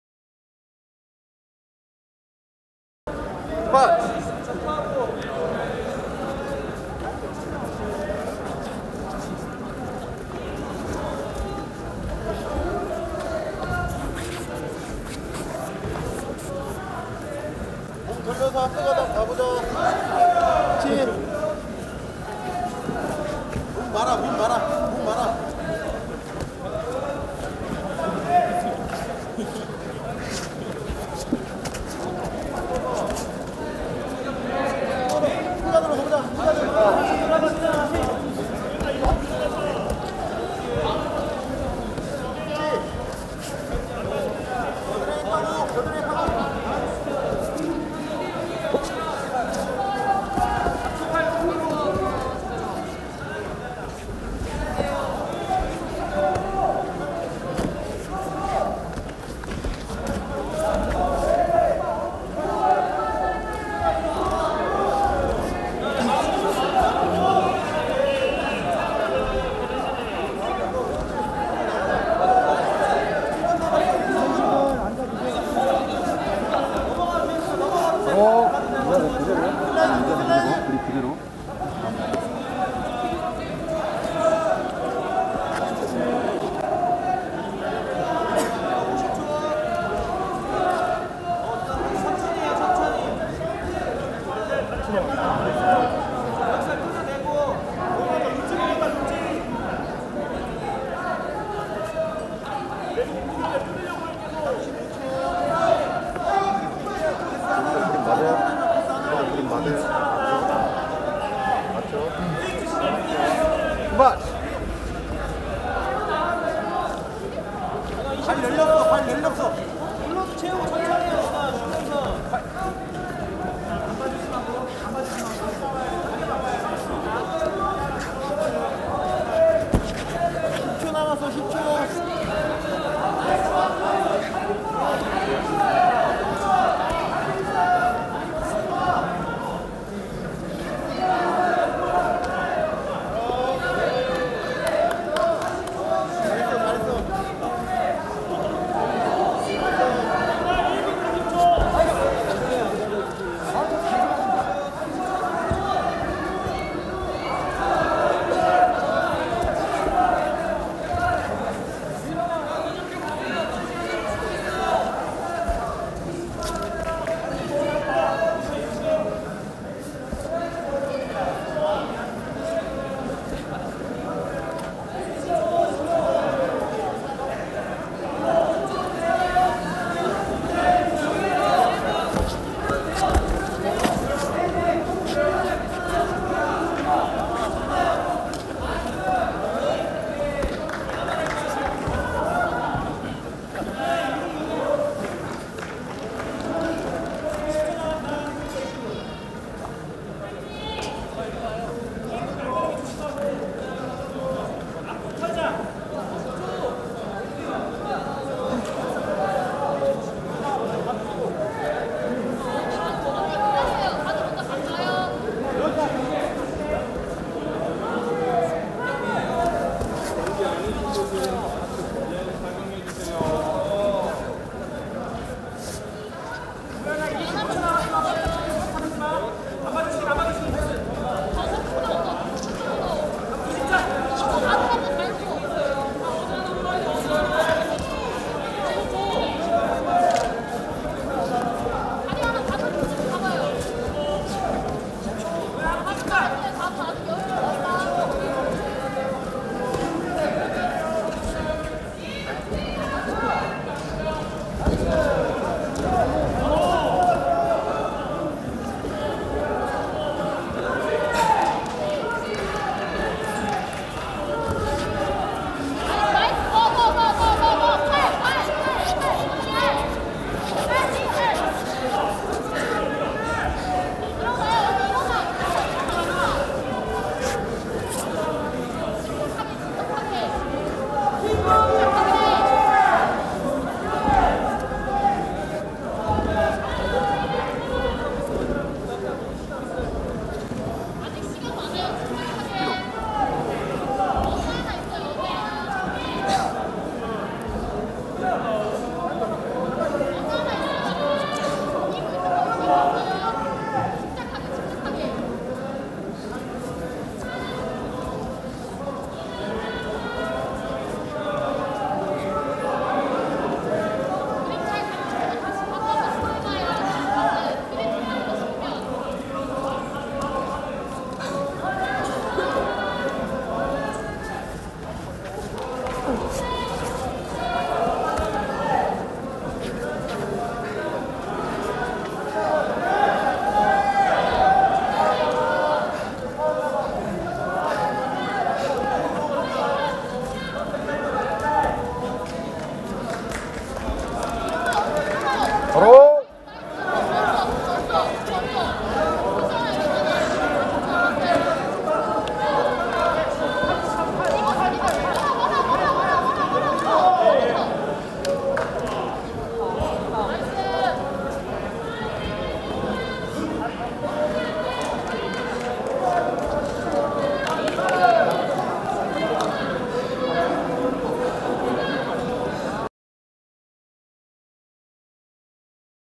제 7회 리얼주짓수 성인부 남자 화이트 -64kg 김명준vs이재선
제 7회 리얼주짓수 우석대학교